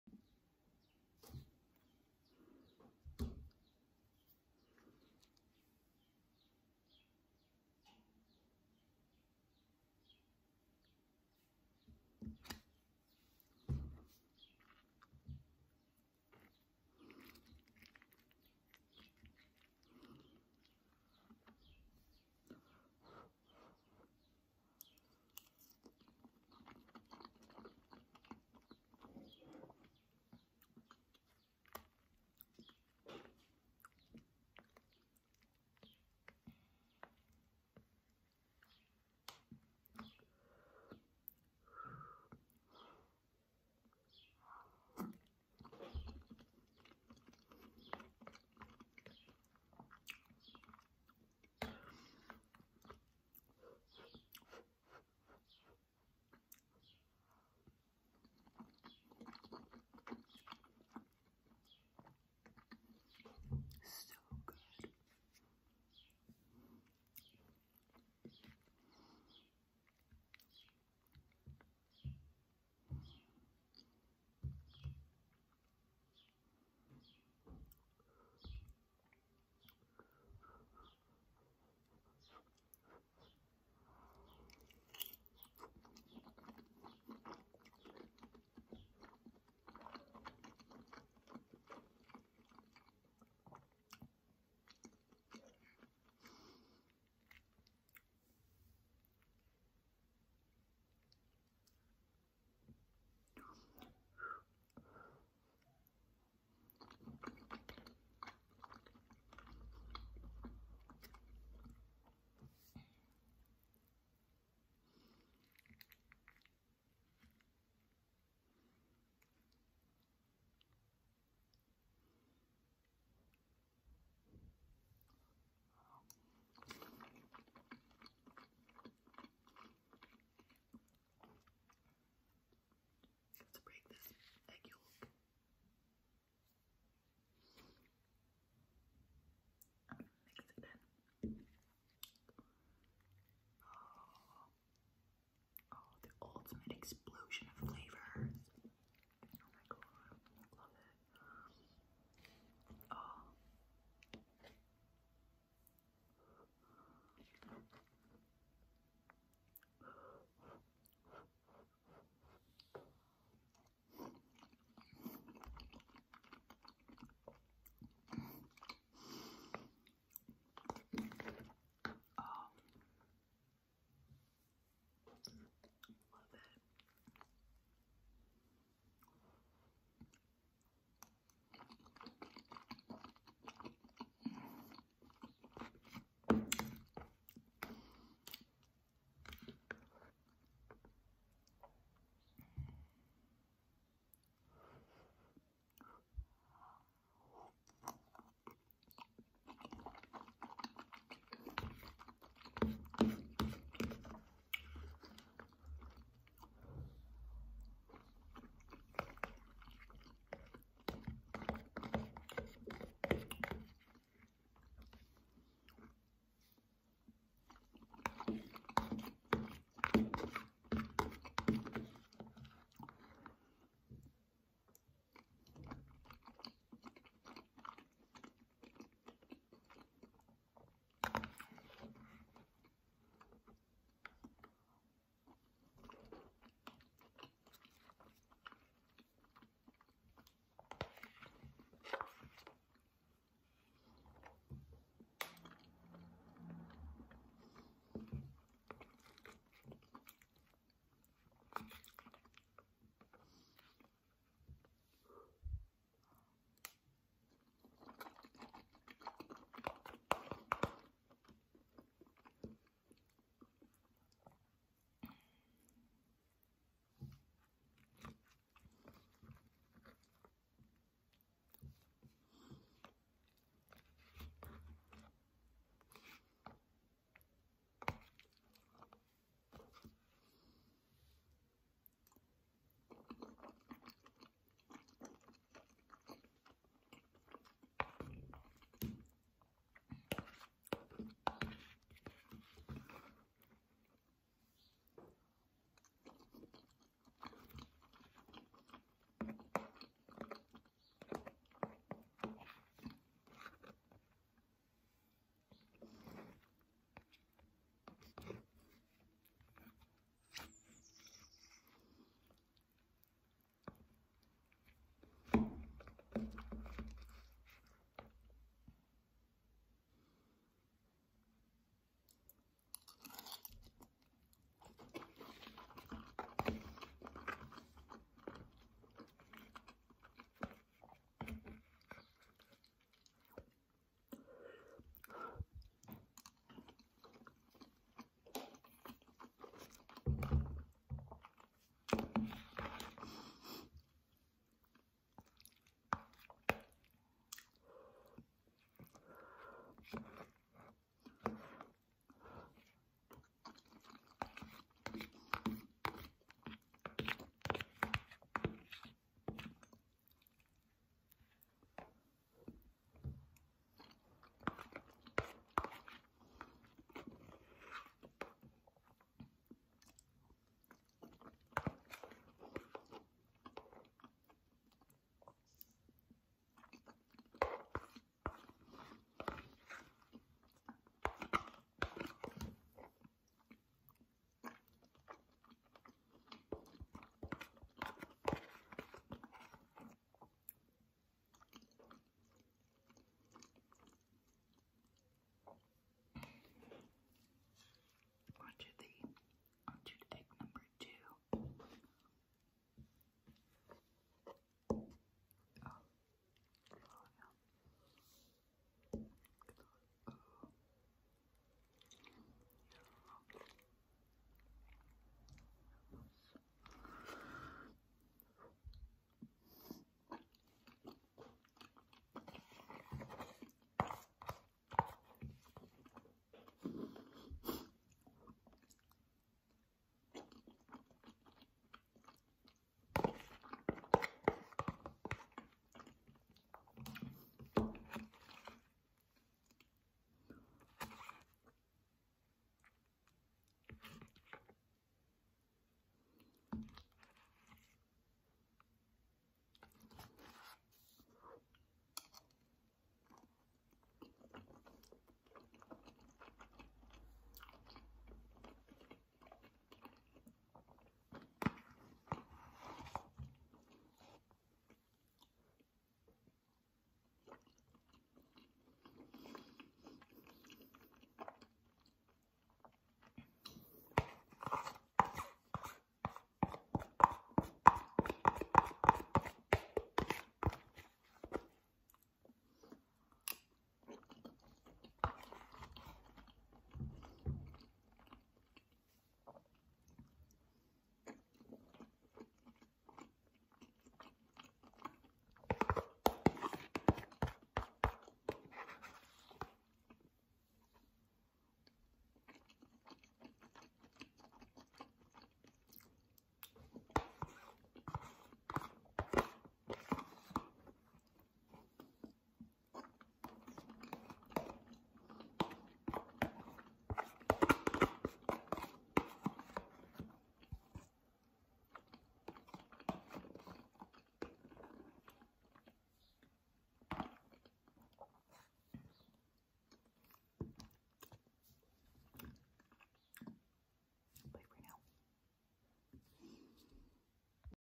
ASMR Mukbang spicy paella leftover with two fried eggs on top
To keep my channel afloat especially if you’d like to continue seeing my uploads, any donations of any size are welcomed!